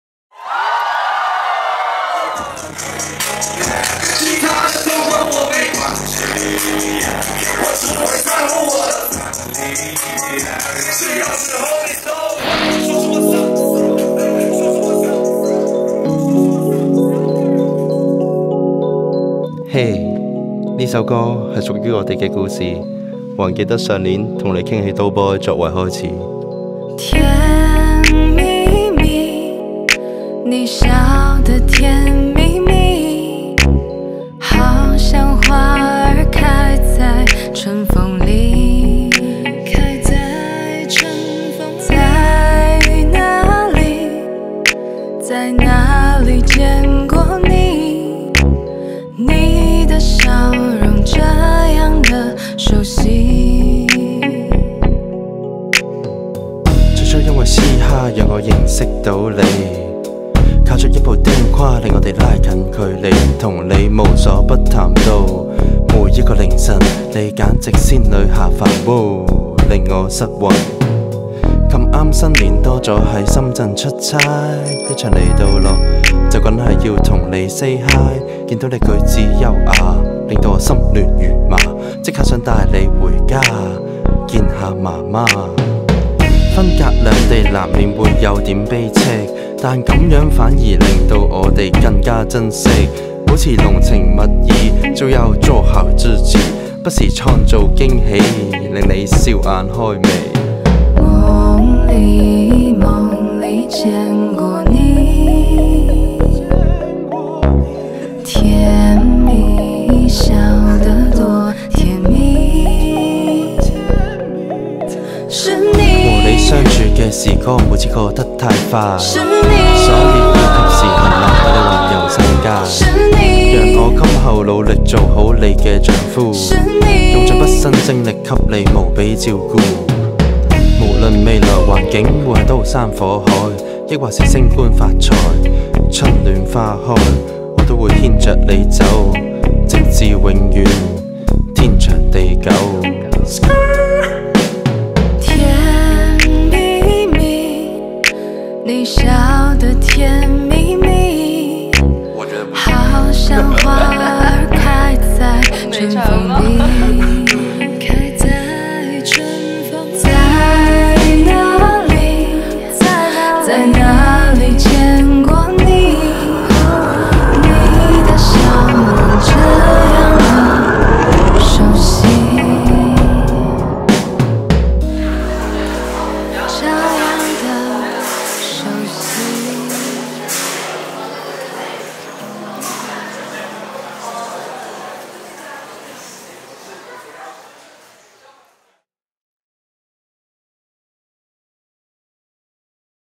「結婚怎可以唔自已作首主題曲」MV --- 屬於我們自己的 HIPHOP RAP FREESTYLE 婚禮早拍晚播！
我們的緣分始於HipHop、始於演出，於是便寫了首這樣的歌。說唱、副歌以及和聲部分全部都是由我們自己演唱的。MV中騎單車和結尾閃回曾經遇到但錯過的畫面則是小小致敬了一下電影《甜蜜蜜》。感謝依興馳和N1FT全體，再感謝王齊銘和霧都哥以及阿桂、Nicole的友情露臉客串。最后感謝MakersFilms的鼓勵讓我們在婚禮前把這首歌趕錄了出來，也感謝他們的用心為我們呈現了一個特別棒的故事。把婚禮的早拍晚播與自創歌曲MV融合在一起，我想對大家來說都是個全新的嘗試，很開心我們做到了！（其實中國傳統的龍鳳褂、利是、金器等等都係超級dope既元素）